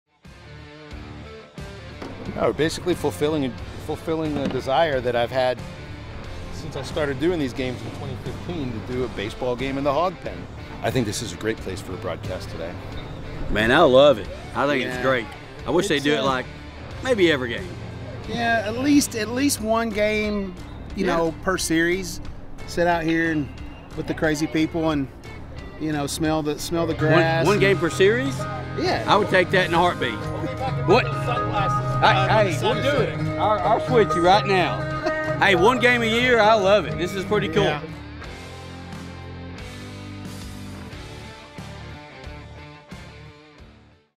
What's it gonna take to get those sunglasses??
Razorback Access: Live from the Hog Pen

It was a special night in the Hog Pen on Tuesday as Phil Elson and Bubba Carpenter took their game broadcast out of the booth and into the crowd. Get a behind-the-scenes look at the Razorback Radio crew's broadcast from the Hog Pen and find out if they plan to do it again.